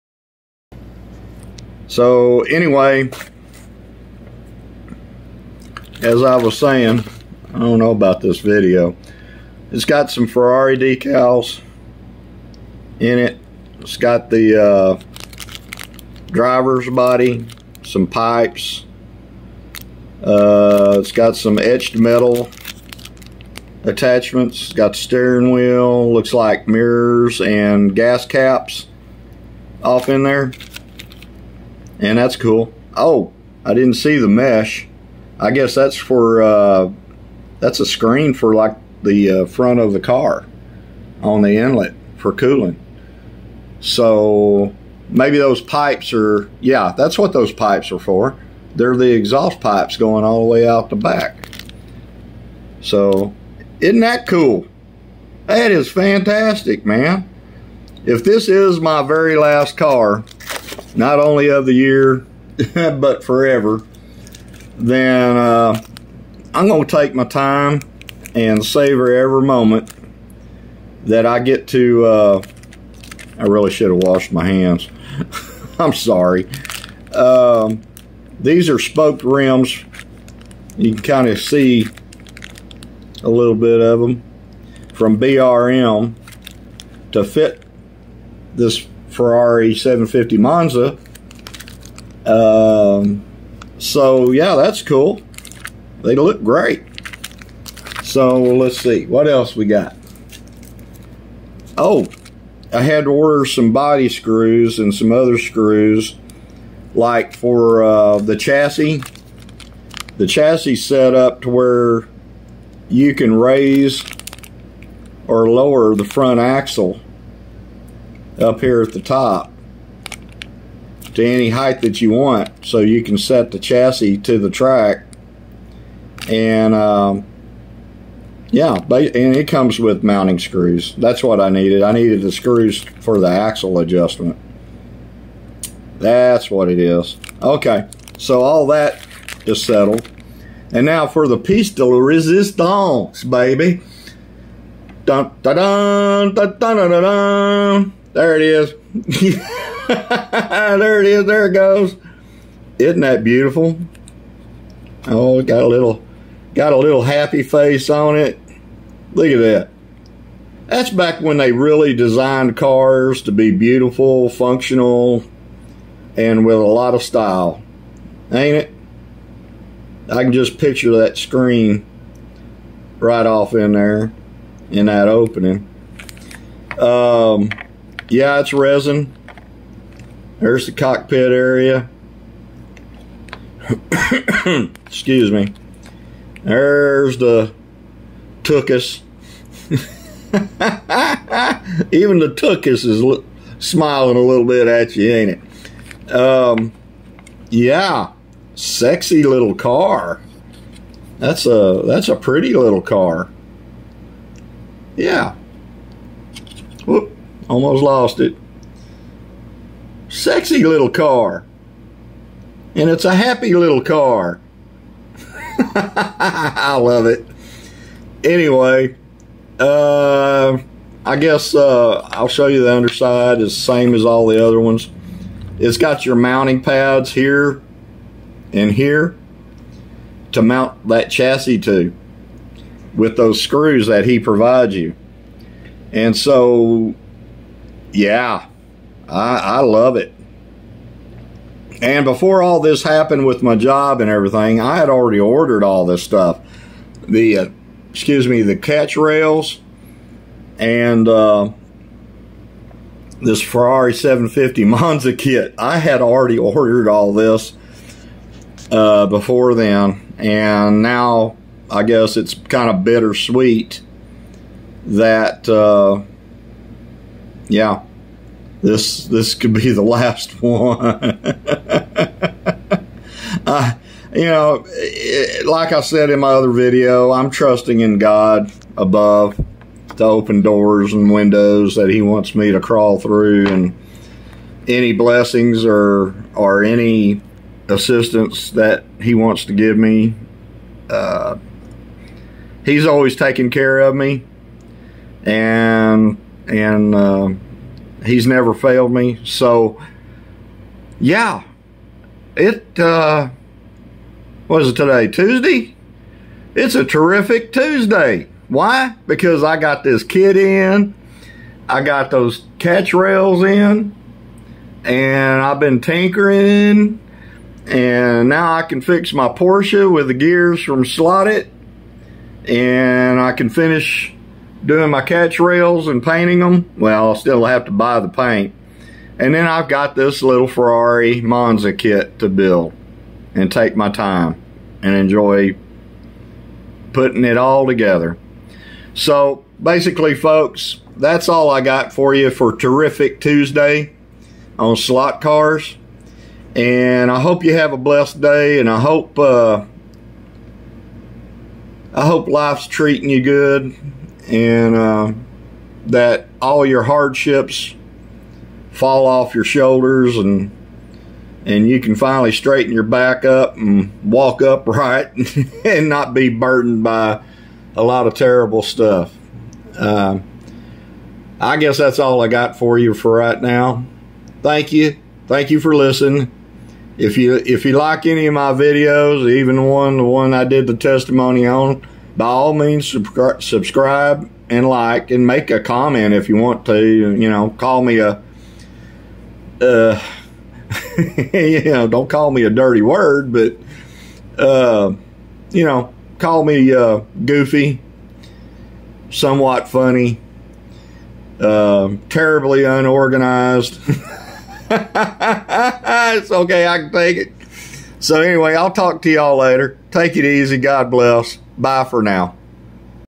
PART 2 of Terrific 👍 Tuesday slot car stuff 😄
😆 lol I don't know what I am doing but hope you have fun with me on what came in the mail today. Slot car stuff 😄